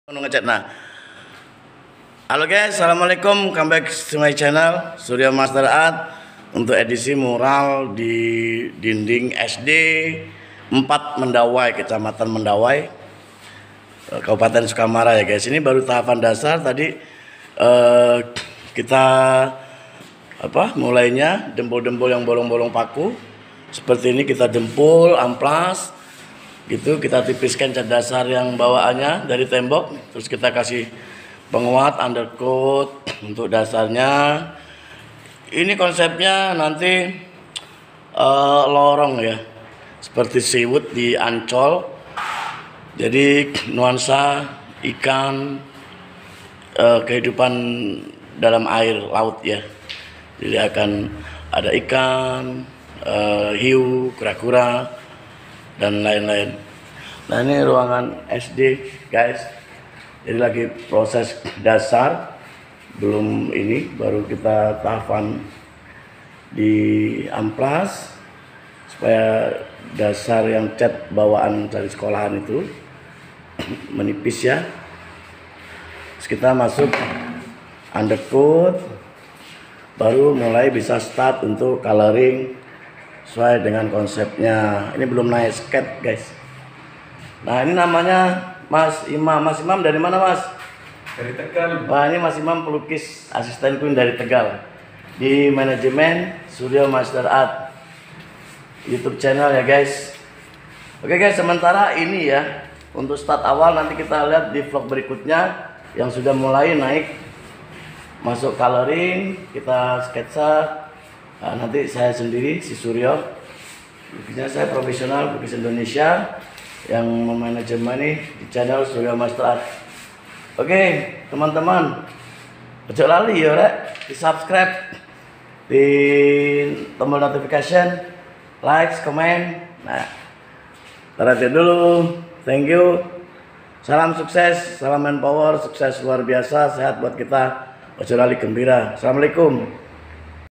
Tahap dasar untuk lukisan mural edukasi ruang kelas SDN Mendawai kab.sukamara th 2022
pekerjaan job arts lukisan mural edukasi ruang kelas SDN Mendawai kab.Sukamara th 2022.Kalinantan Tengah.untuk yg tertarik  dgn lukisan mural dinding hubungi management channel YouTube SURYO MASYER ATRS . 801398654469